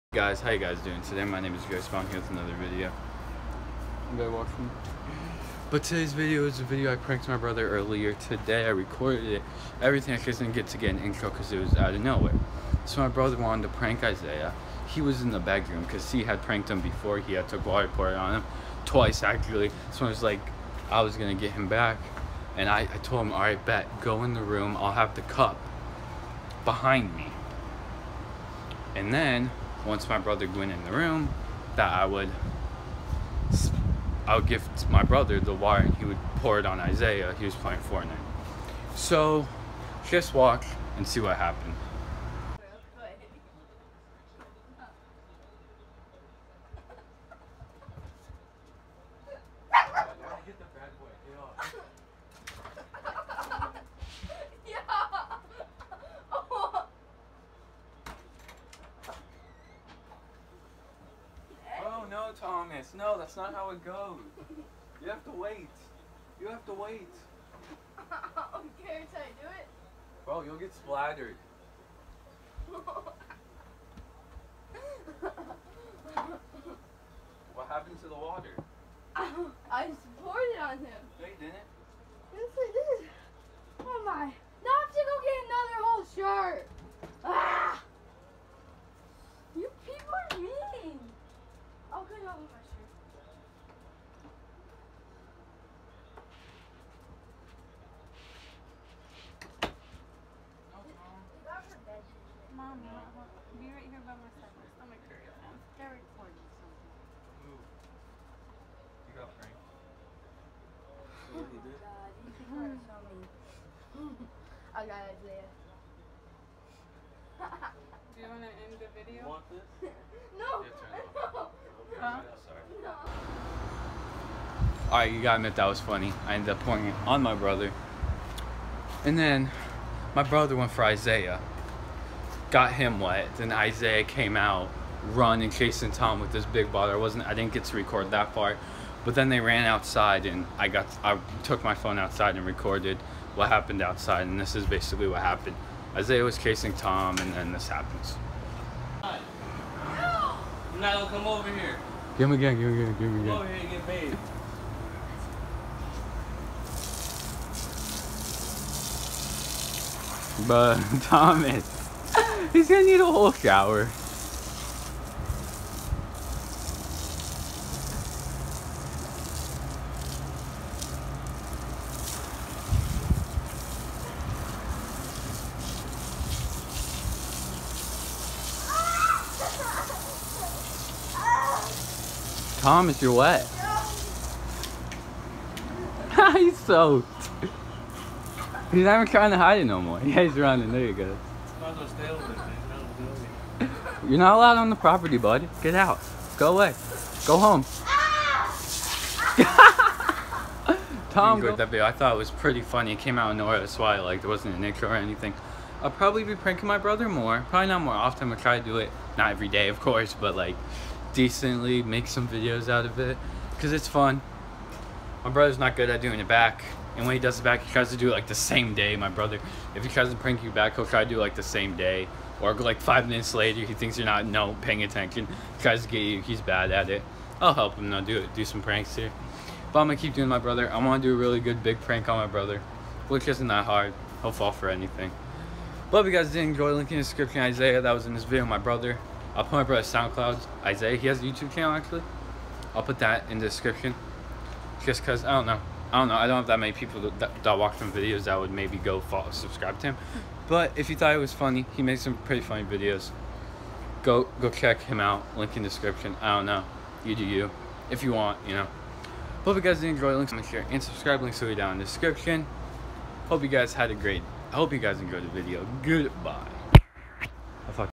We pranked my little brother!!!!!!
This is a short video of me and my dog praking my brother!

Snap Chat - jspano44